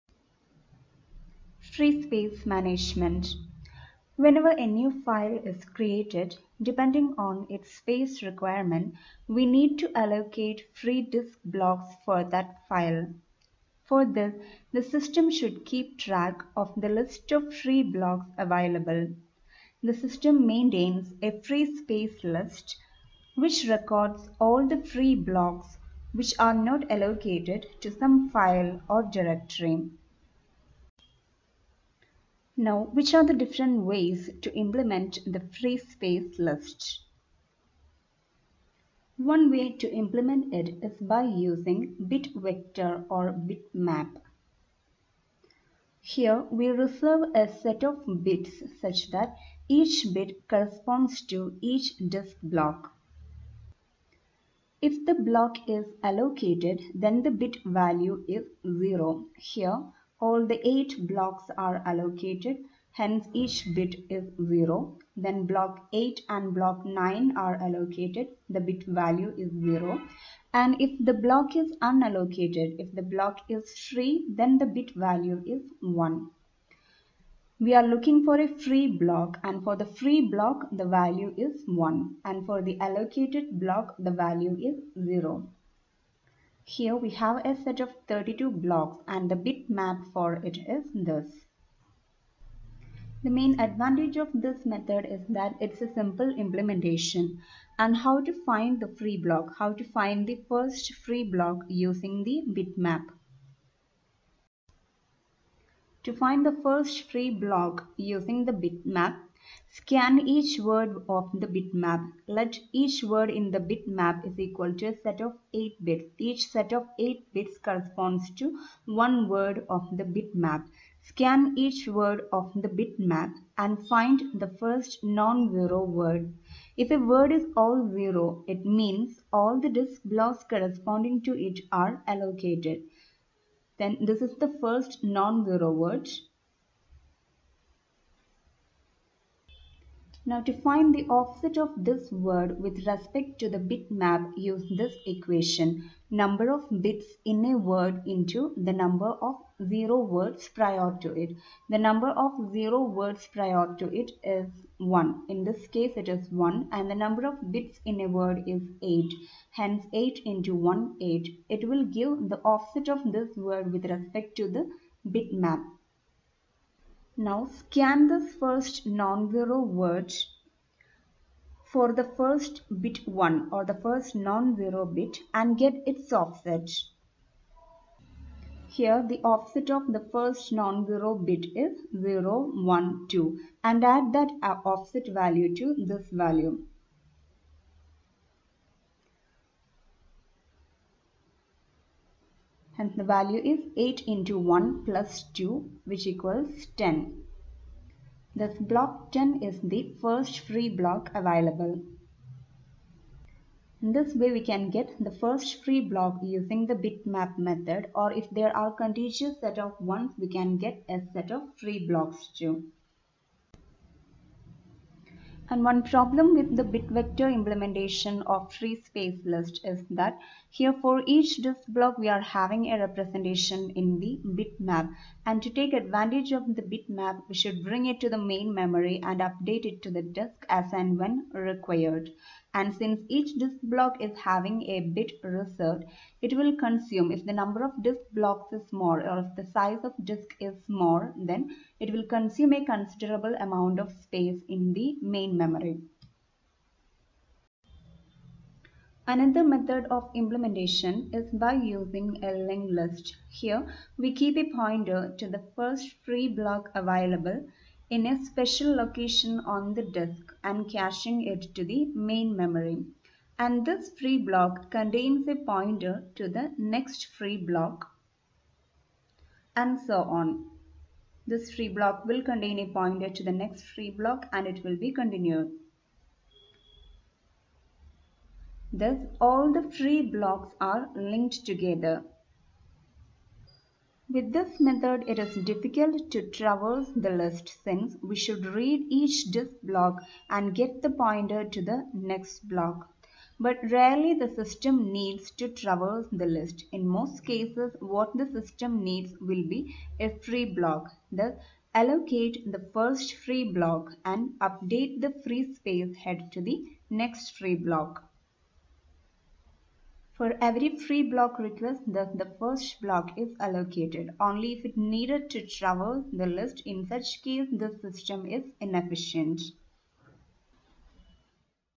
Free Space Management-Tutorial12
To understand free space management and how to implement free space list using bitvector and linkedlist